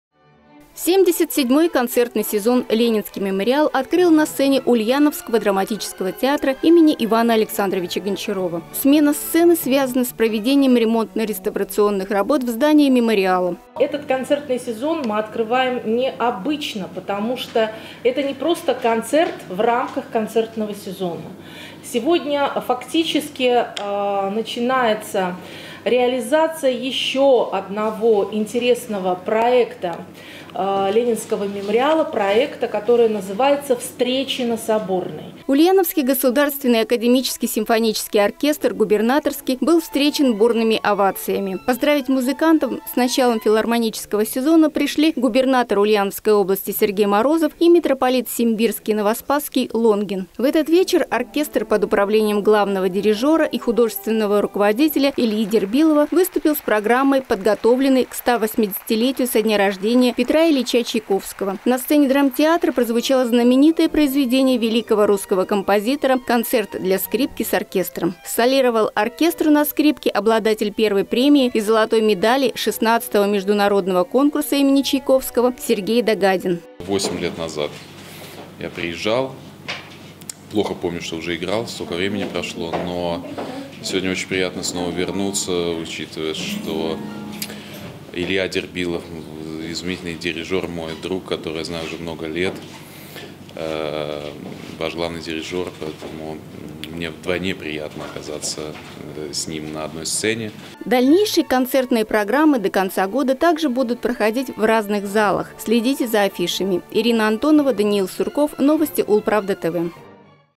Новая сцена и новый проект. Ленинский мемориал торжественно открыл концертный сезон
77-й концертный сезон Ленинский мемориал открыл на сцене Ульяновского драматического театра им. И.А. Гончарова. Смена сцены связана с ремонтно-реставрационными работами в здании мемцентра.